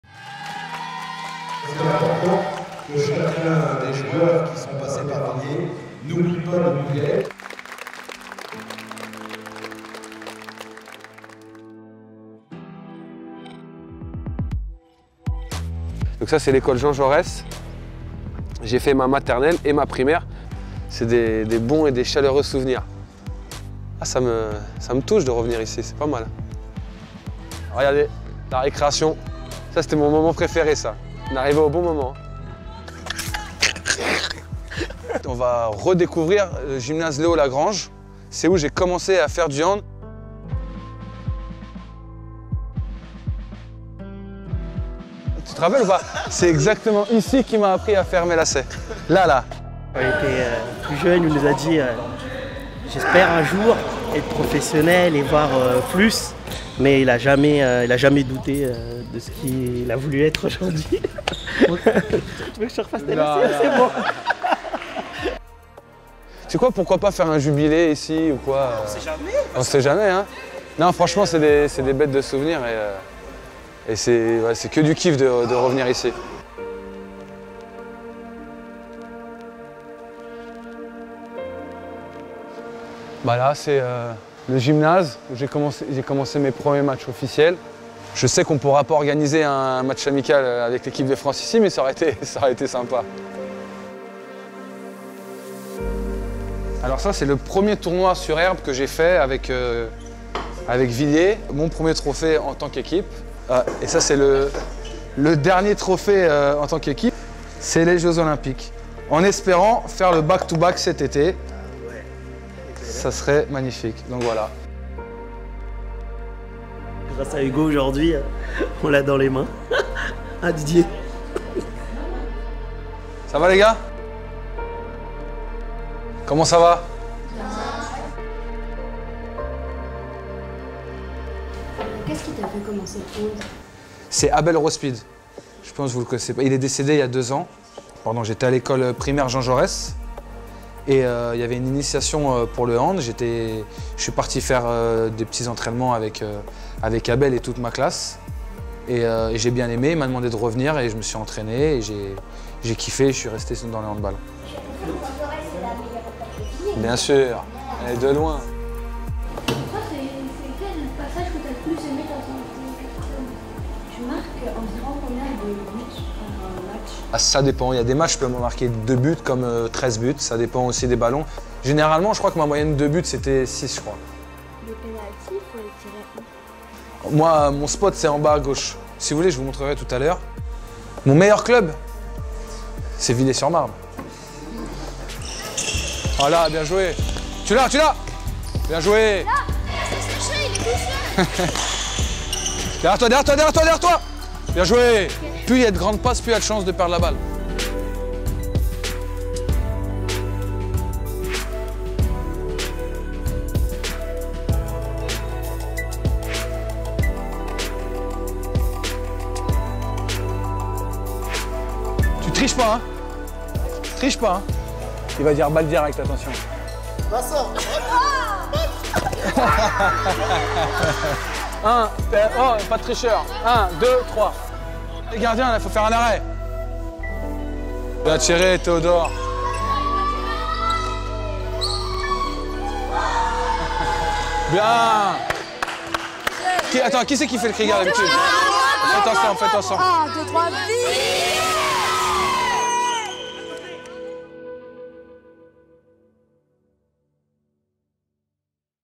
Sur les traces d'Hugo Descat 💪
Retour au club de Villiers-sur-Marne, club où l'international français a fait ses premiers pas ballon en mains 🤾‍♂️
Retour sur un échange enrichissant entre Hugo et les jeunes handballeurs du club de Villiers-sur-Marne 🙌
Si comme Hugo, tu souhaites rejoindre la grande famille du handball, rendez-vous pour trouver le club le plus proche de chez toi !